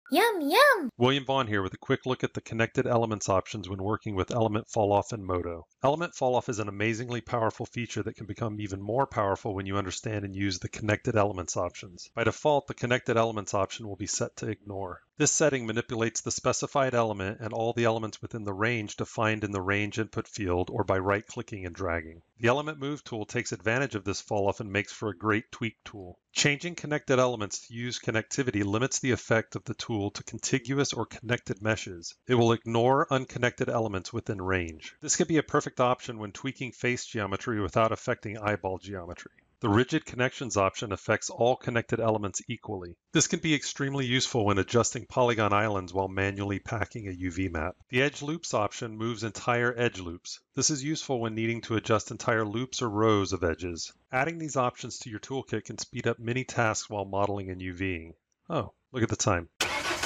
MODO | Connected Elements Options for Element Falloff
Element Falloff is an amazingly powerful feature that can become even more powerful when you understand and use the Connected Elements options. This video takes a quick look at the Connected Elements Options when working with Element Falloff in MODO.